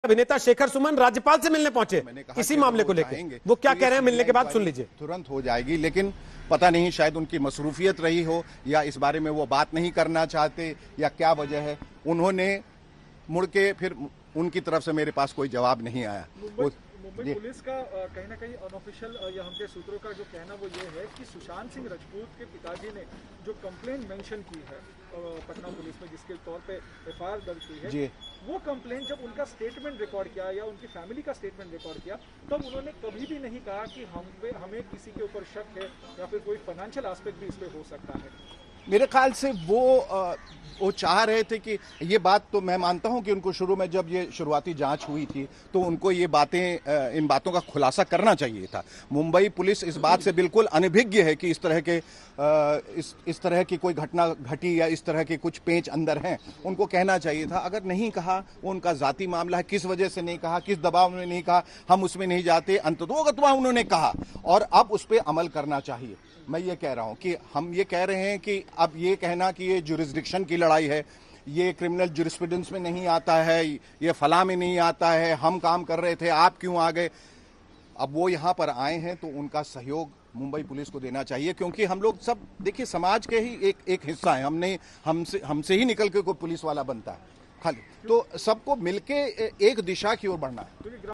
Sushant Death Case : Shekhar Suman ने की Maharashtra के राज्यपाल से मुलाकात
सुशांत सिंह राजपूत मामले में जांच की प्रक्रिया दिनोंदिन बढ़ती जा रही है. इस बीच कई नेताओं से लेकर बॉलीवुड कलाकारों ने सुसाइड मामले की जांच सीबीआइ से कराने की मांग की है. पटना में पिता के द्वारा सुशांत केस की एफआइआर दर्ज होने पर खुशी जाहिर करने वाले अभिनेता शेखर सुमन ने एक बार फिर सीबीआइ जांच की मांग दोहराई है. इसके लिए शुक्रवार की दोपहर शेखर महाराष्ट्र के राज्यपाल भगत सिंह कोश्यारी से मिले.